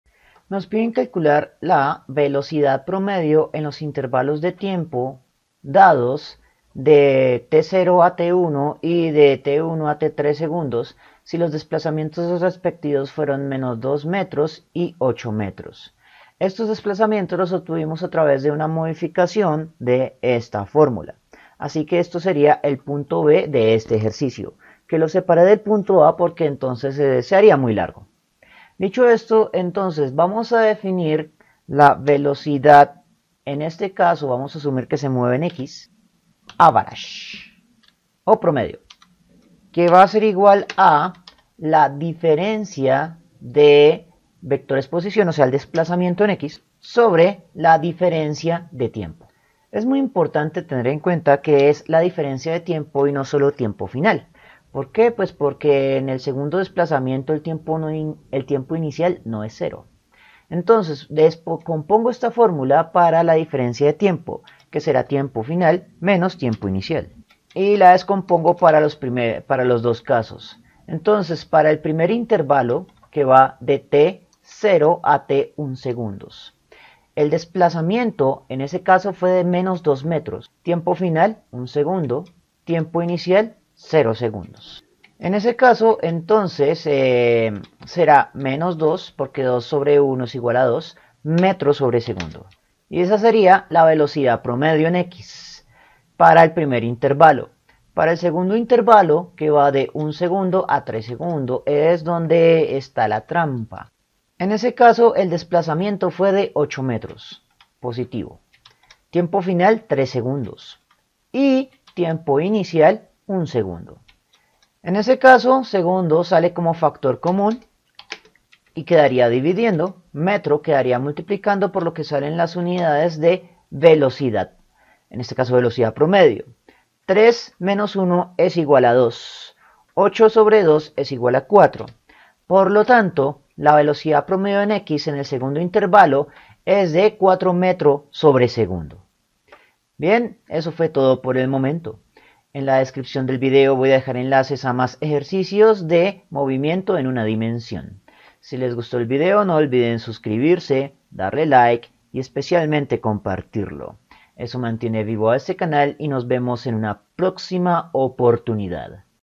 Calcular la 𝐯𝐞𝐥𝐨𝐜𝐢𝐝𝐚𝐝 𝐩𝐫𝐨𝐦𝐞𝐝𝐢𝐨 Si 𝒕 = 0 a 𝒕 = 1 𝐬 y 𝒕 = 1 𝐬 a 𝒕 = 3 𝐬 𝑺𝟏 =-2 𝐦 y 𝒔𝟐 = 8 𝐦
👉 𝑬𝒏𝒖𝒏𝒄𝒊𝒂𝒅𝒐: Una partícula se mueve a lo largo del eje x. Su posición varía con el tiempo según la expresión x = -4t + 2t2, donde x está en metros y t en segundos. (B) Calcule la velocidad promedio en los intervalos de tiempo t = 0 a t = 1 s y t = 1 s a t = 3 s, si los desplazamientos respectivos fueron -2 m y 8 m.